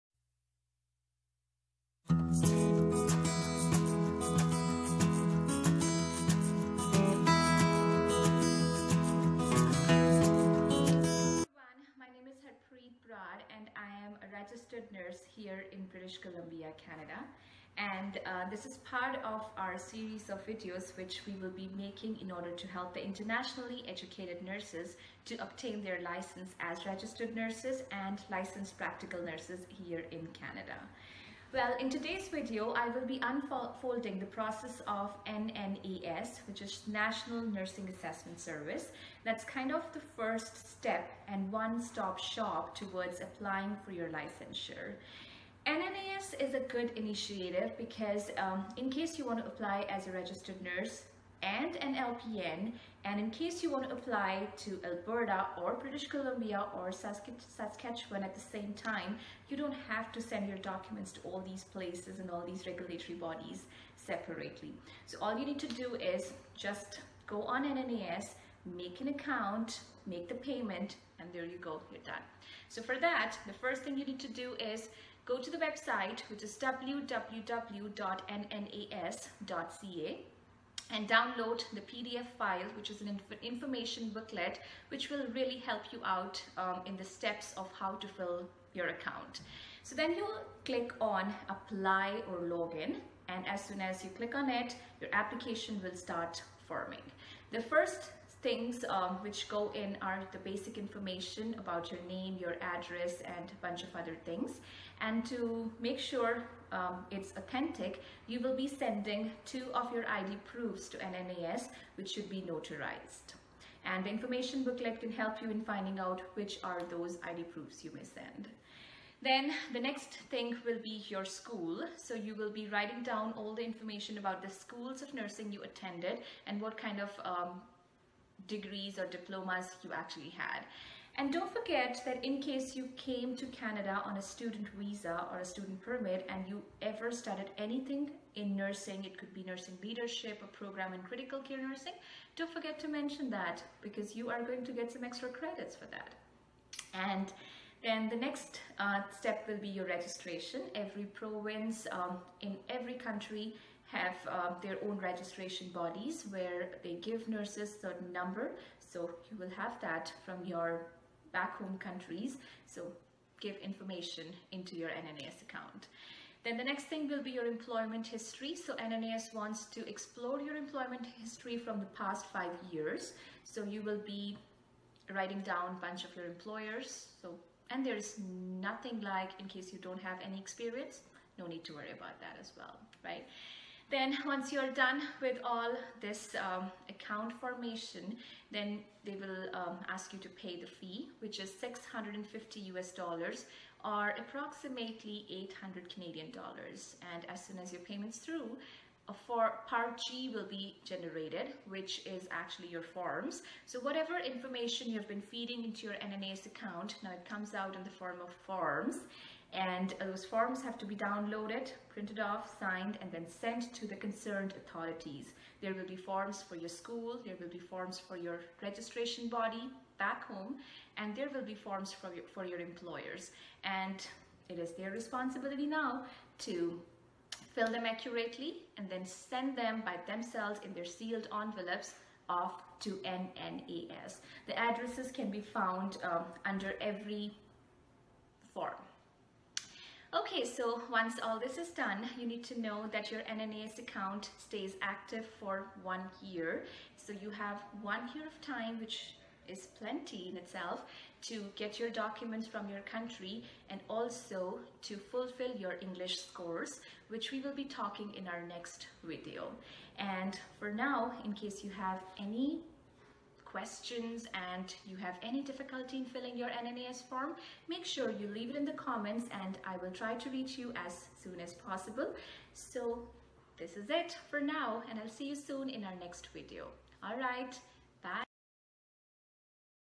Nursing in Canada for Internationally Educated Nurses Part 1
The first step in your application is NNAS.
In this video, I have tried to describe the steps for NNAS.